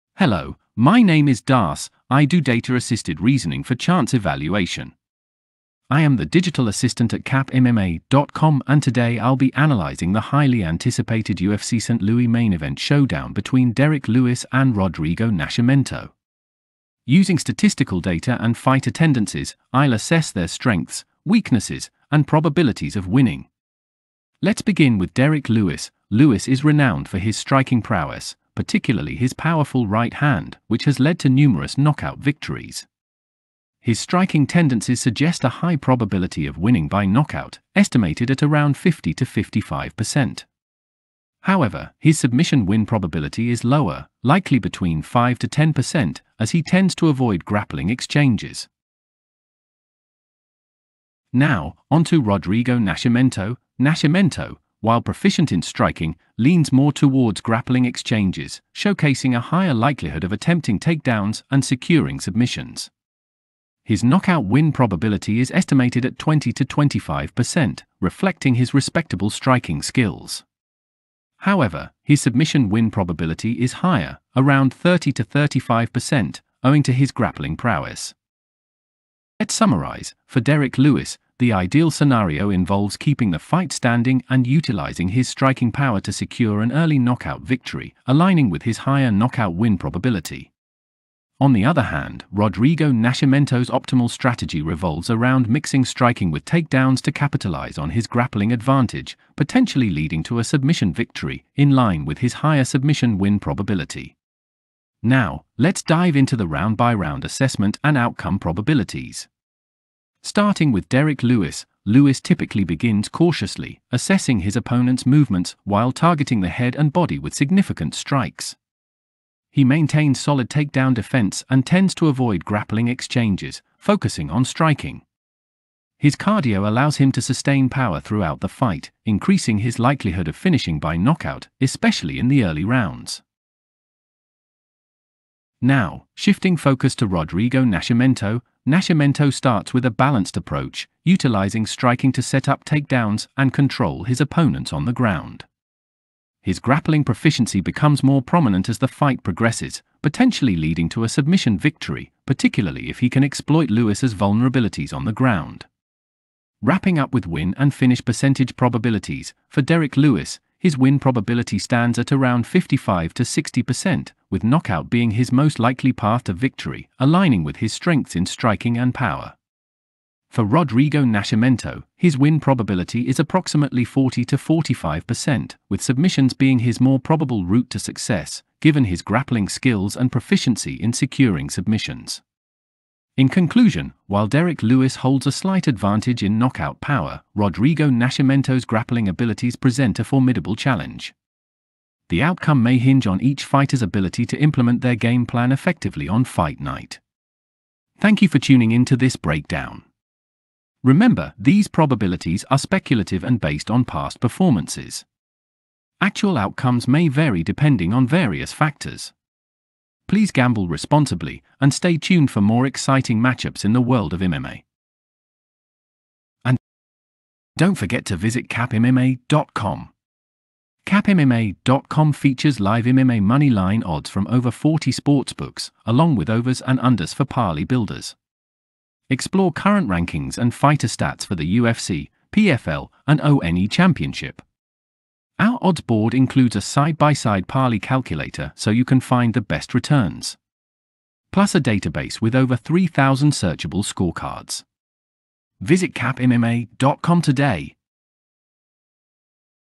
UFC St. Louis Lewis vs Nascimento Fight GPT Prediction
DARCE is a Data-Assisted Reasoning for Chance Evaluation (DARCE) bot. Today, DARCE will be dissecting the UFC St. Louis's main event featuring Derrick Lewis vs Rodrigo Nascimento. Utilizing statistical data from both Lewis and Nascimento's last five fights, DARCE will provide a round-by-round breakdown of their tendencies in striking, grappling, and overall output. Following this analysis, I'll offer percentage probabilities for each fighter's win outcome.